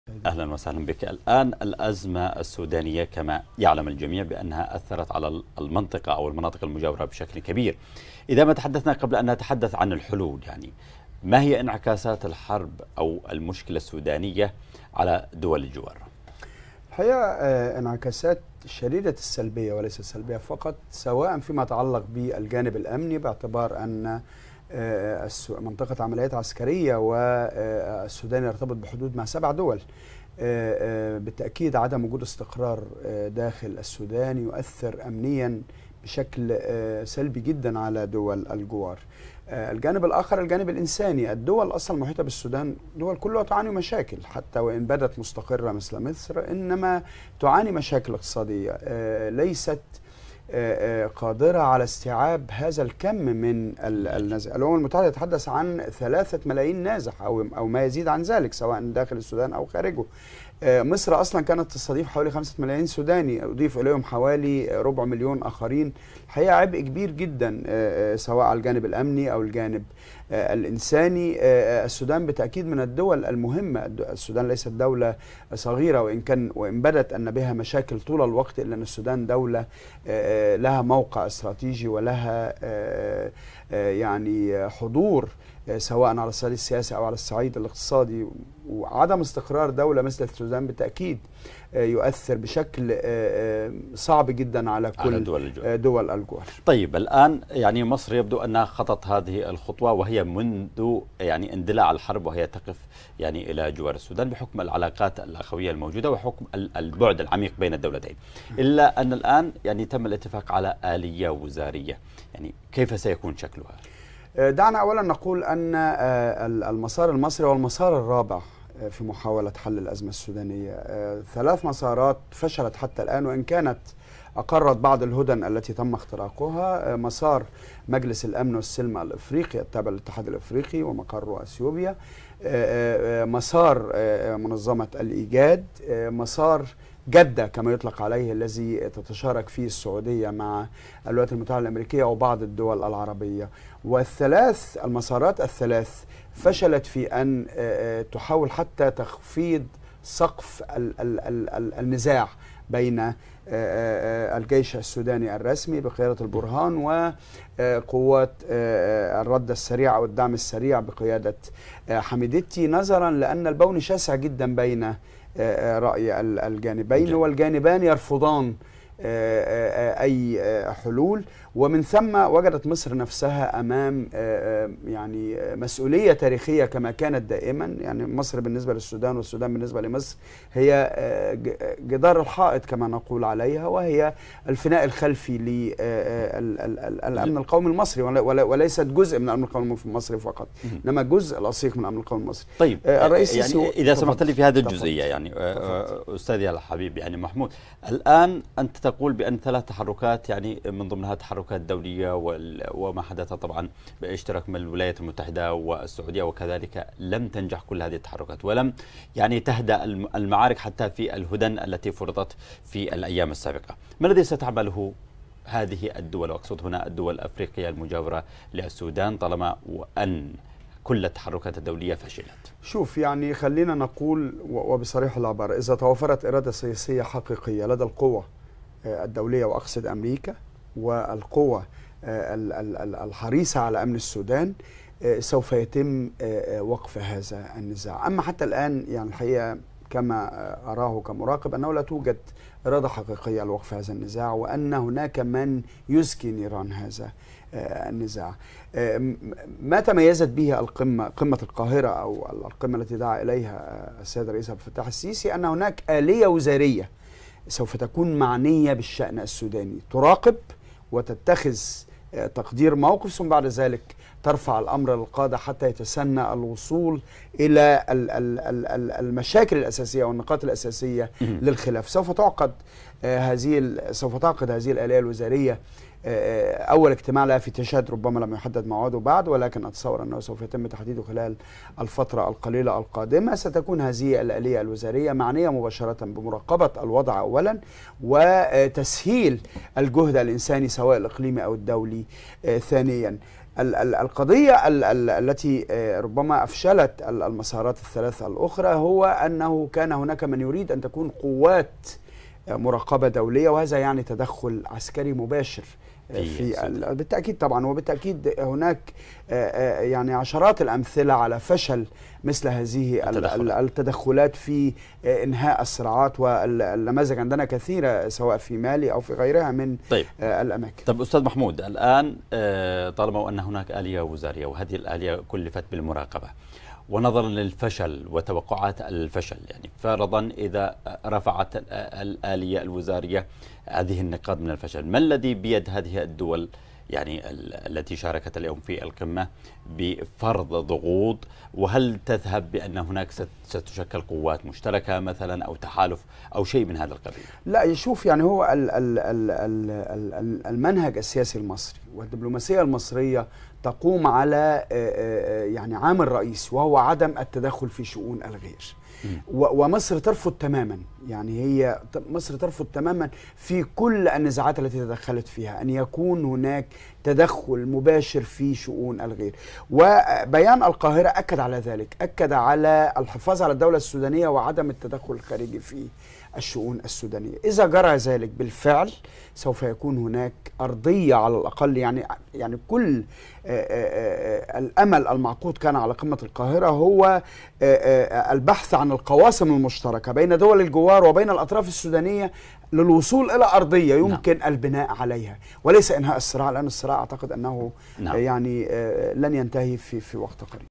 محمود الشناوي: المصرية عدم استقرار السودان له تأثير سلبي على دول الجوار
مدير تحرير وكالة أنباء الشرق الأوسط

تليجرام